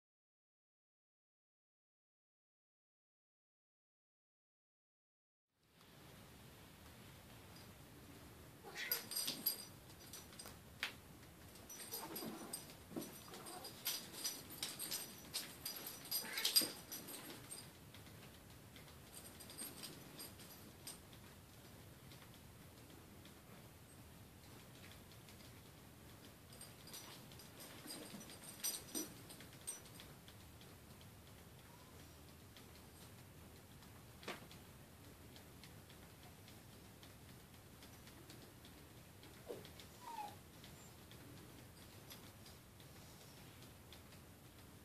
Foster Dog Whipple & Rescued Cat Play Together
Our foster dog Whipple and rescued kitty playing together